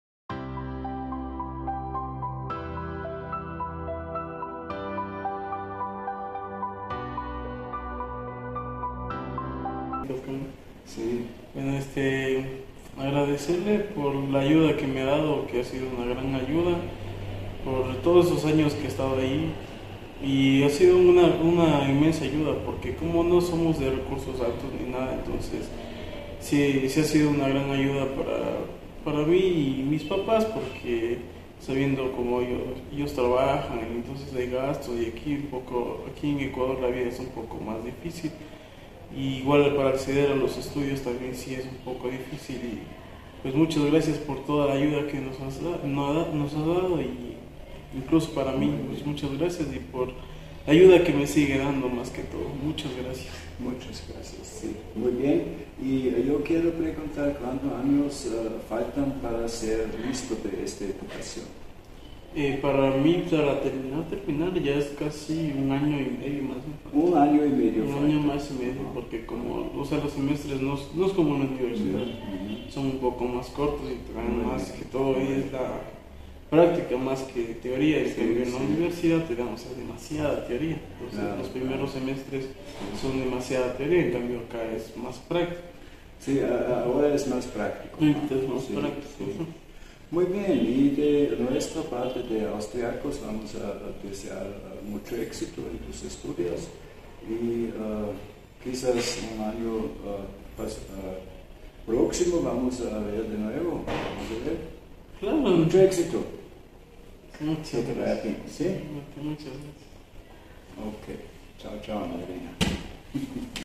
Gabriel 1
Gabriel kann nach seinem Maturaabschluss durch die Hilfe seiner österrreichischen Patin eine Ausbildung zum Diplomkrankenpfleger machen.